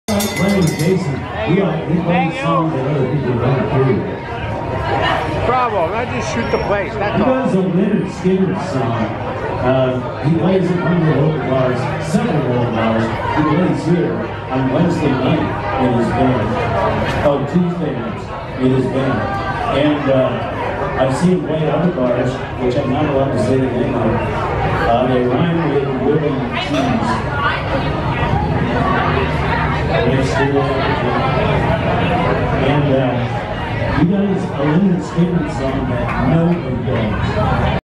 Ringing of the ship's bell at Sloppy Joe's, Key West
Tips are acknowledged by the Sloppy Joe's bartenders by the ringing of the bell.